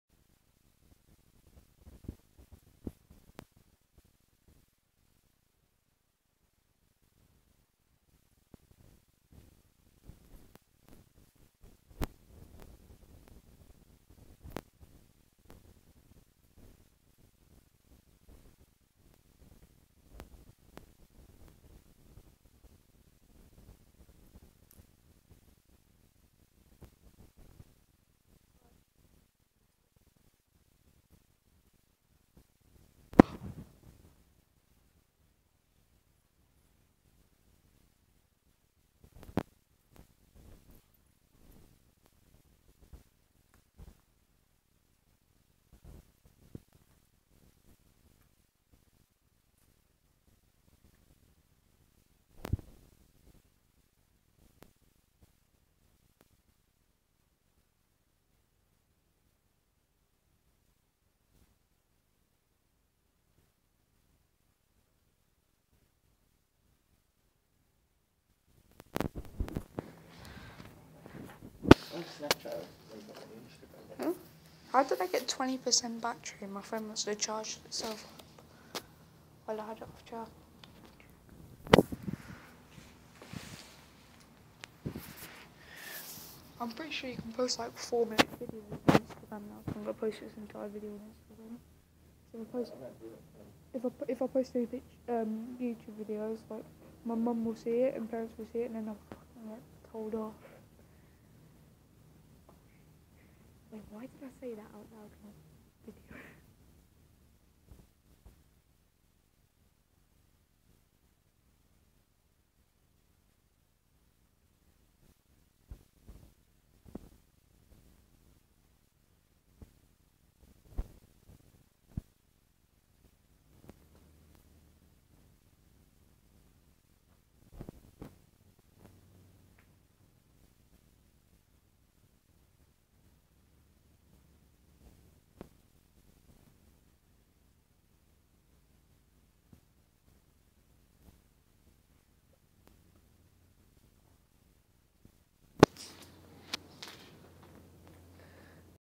St Crispin's Mental Asylum
So on September 1st I went to this mental asylum and this 3rd time I've uploaded it cause of I used copyrighted music